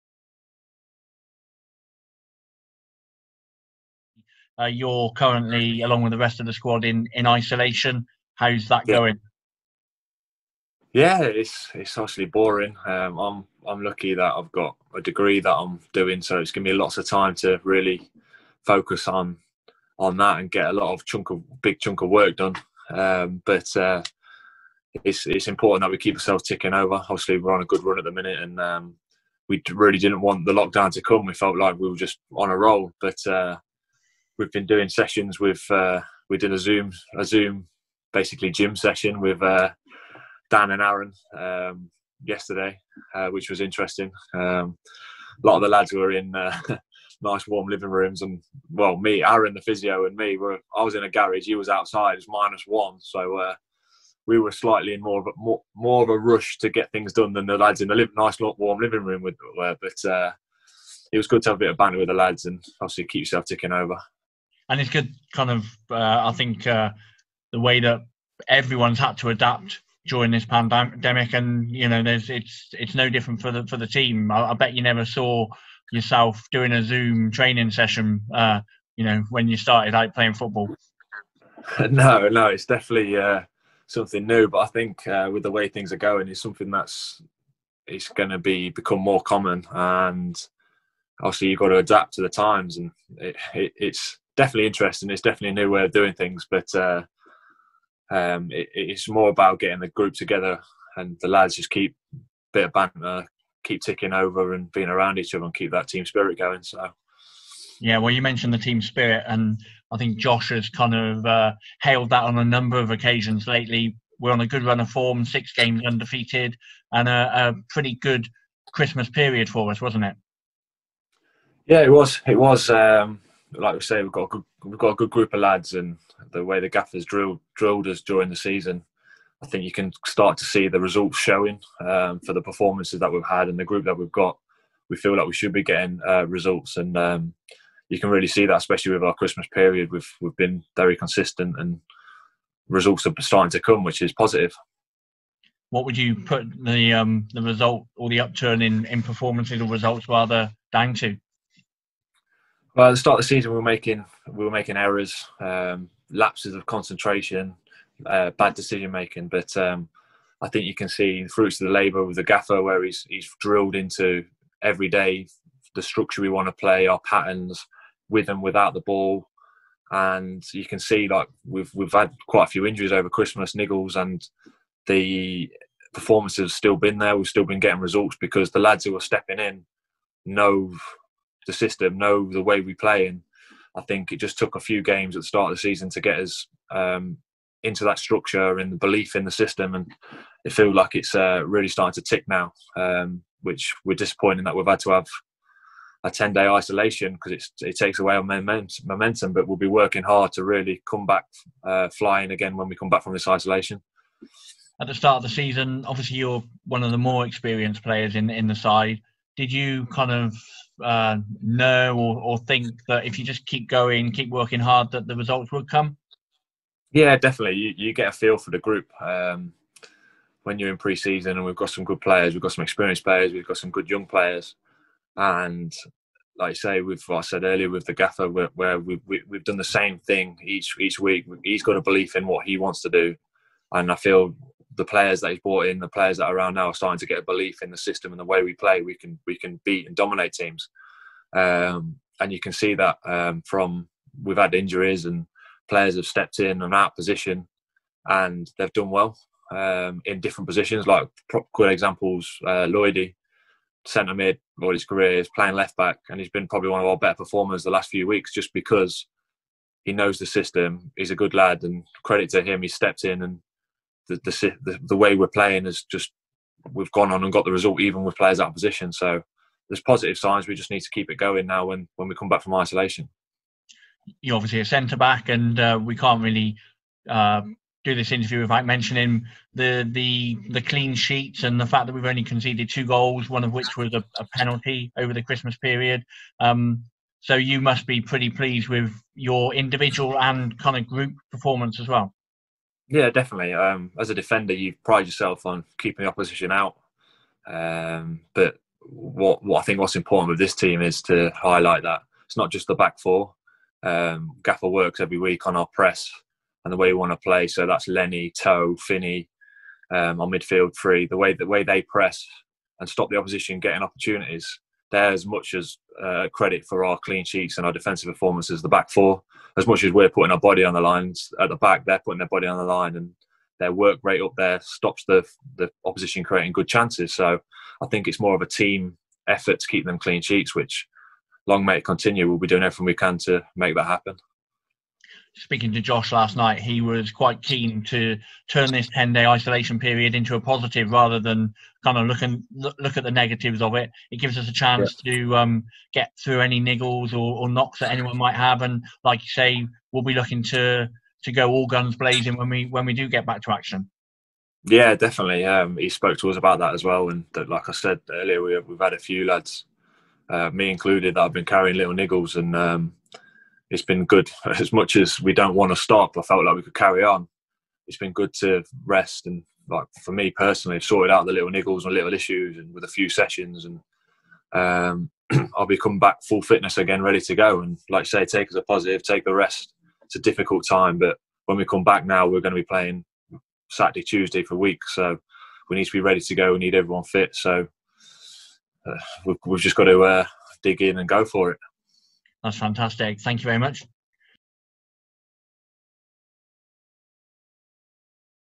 INTERVIEW | Grimes Reflects On Season So Far
Jamie Grimes reflects on the season so far.